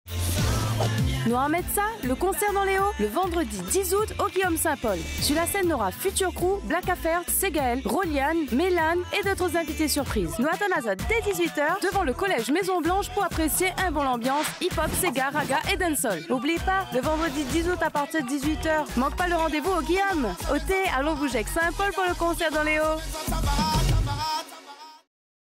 Concert dan lé O 2012
Nou armèt sa, le Concert dan lé Ô, le vendredi 10 août 2012 au Guillaume Saint-Paul !

Su la scène nora : FUTUR CREW, BLACK AFFAIR, SEGAEL, ROLIAN', MAYLAN et d'autres invités surprise !

Nou attend à zot dé 20h devant le collège Maison Blanche pou apprecier in bon l'ambians' hip-hop, séga, ragga et dancehall.

Oubli pa ! le vendredi 10 août à partir de 20h, manque pas le rendez-vous au Guillaume !

Oté ! Allon bouge ék Saint-Paul pou le concert dans les O !